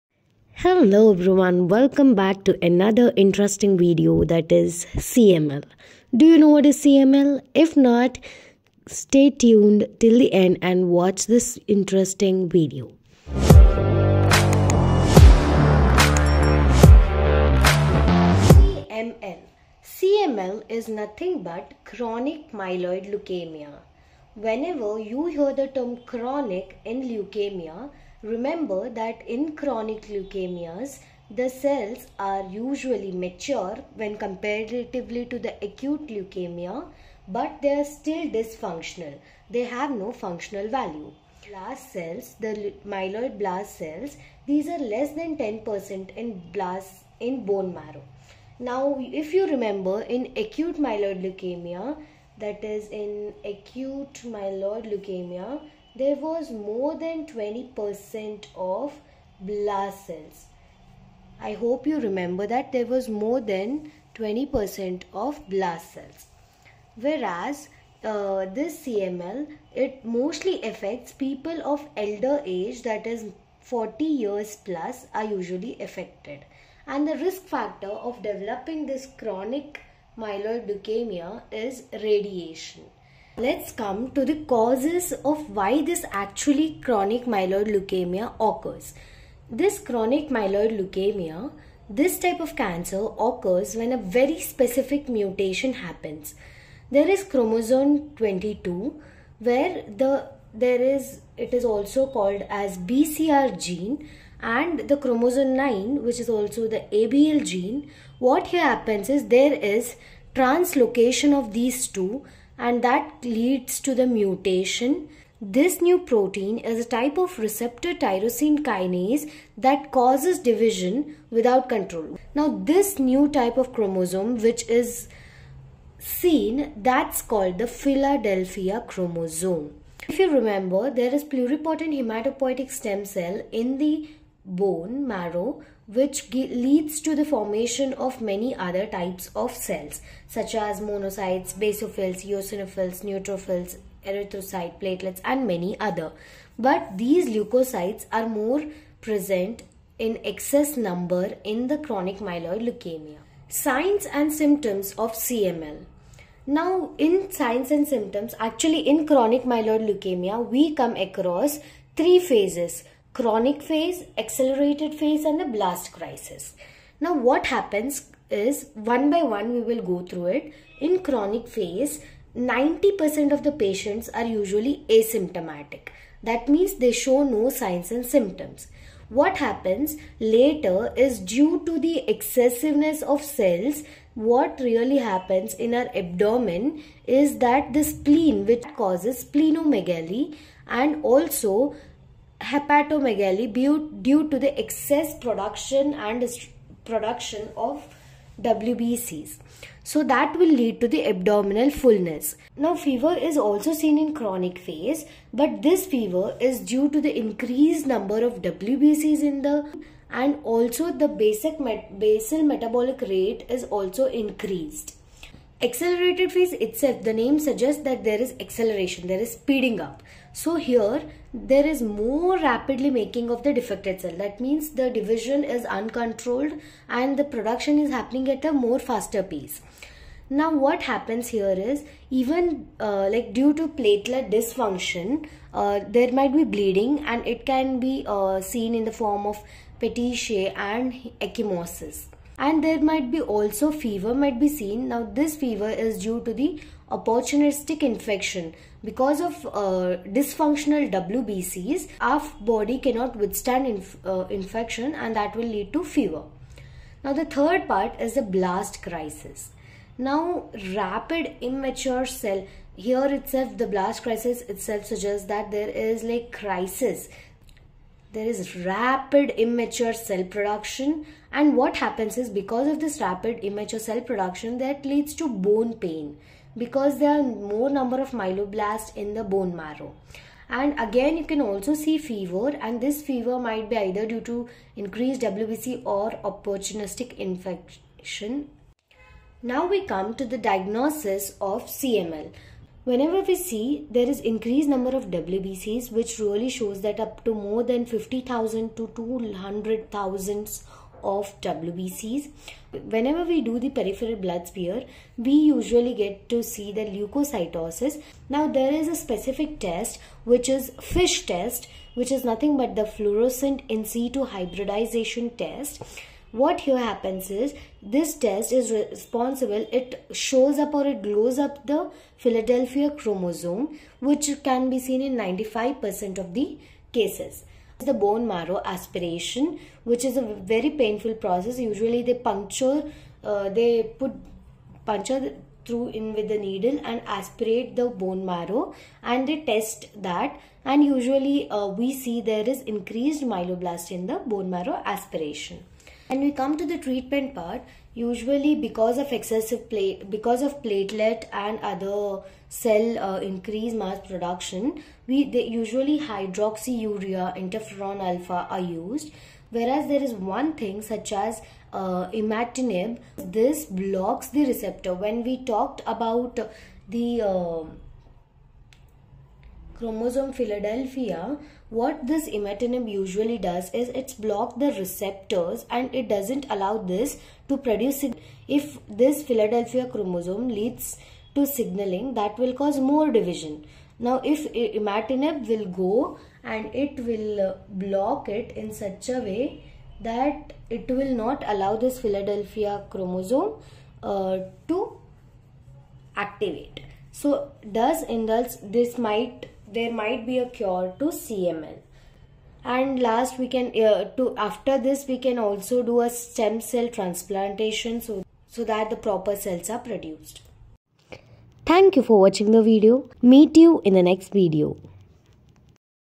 What is Chronic Myeloid Leukemia? | Symptoms and Diagnosis
"Understanding Chronic Myeloid Leukemia: Symptoms, Diagnosis, and Treatment"
 In this informative lecture, we delve into the complex world of chronic myeloid leukemia (CML) - a rare type of blood cancer that affects people of all ages. Our expert speaker will discuss the symptoms of CML, how it is diagnosed, and the various treatment options available.

You'll learn about the biology behind CML, including the genetic mutation that causes it, and how it progresses over time. We'll explore the different stages of CML and how they are managed, including targeted therapies and bone marrow transplants.

Whether you are a patient, caregiver, or simply interested in learning more about this disease, this lecture provides a comprehensive overview of CML and the latest advances in treatment. Join us as we increase awareness and understanding of this often-misunderstood condition.